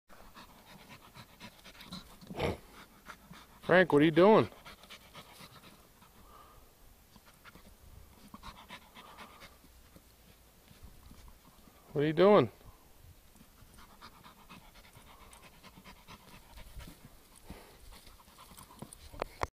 Lazy Lab Digging
Our Black Lab Frank showing how to dig without using too much energy...